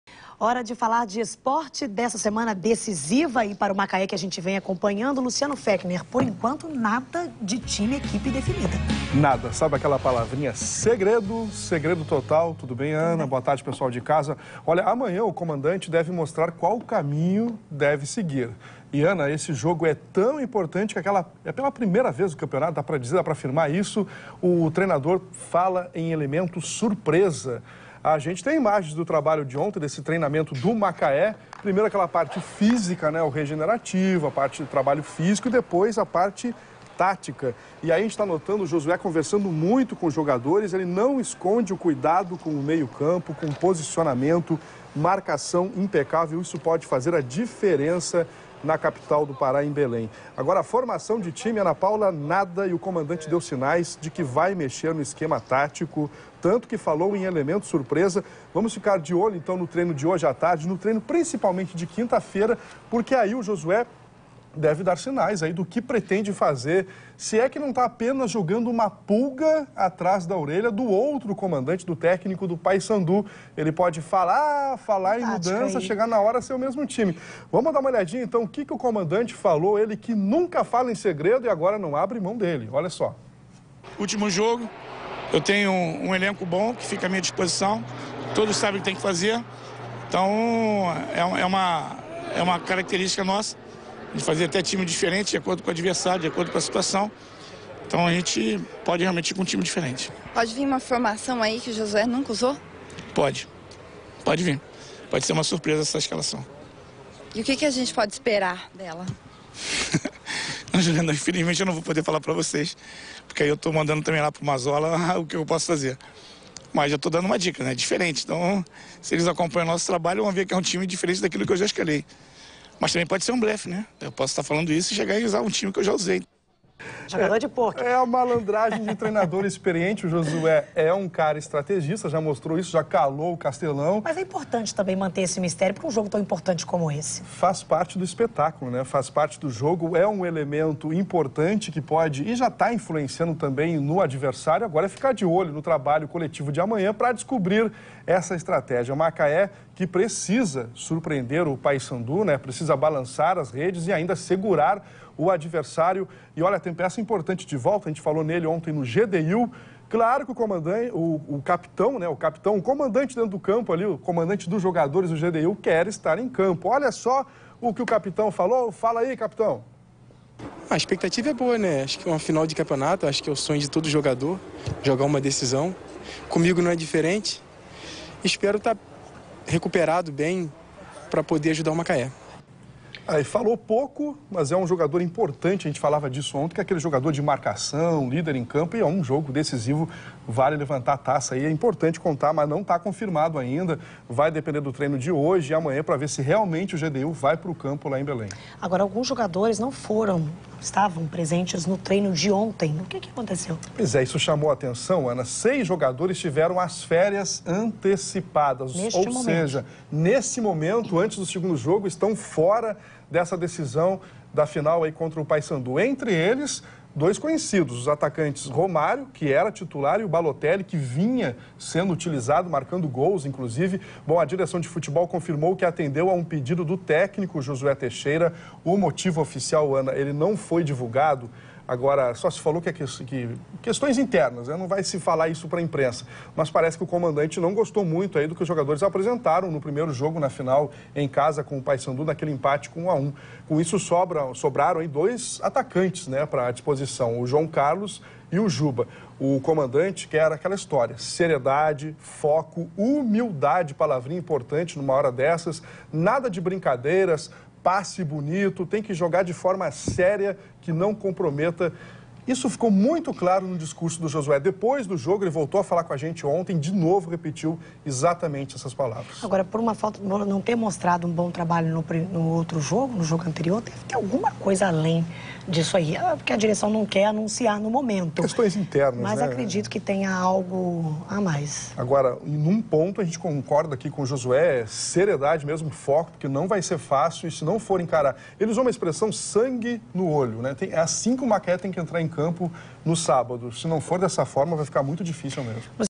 Técnico do Macaé promete surpresa para o jogo de sábado
De todos os setores do Macaé, o ataque certamente foi aquele mais prejudicado pelo afastamento dos jogadores esta semana. Isso porque, dos seis atletas que tiveram suas férias antecipadas, três são atacantes: Romário, Jonathan Balotelli e Diniz. Para a segunda e decisiva partida contra o Paysandu, válida pela final da Série C do Brasileirão, a equipe alvianil tem no elenco apenas três jogadores na posição. São eles Juba, João Carlos e Leozinho.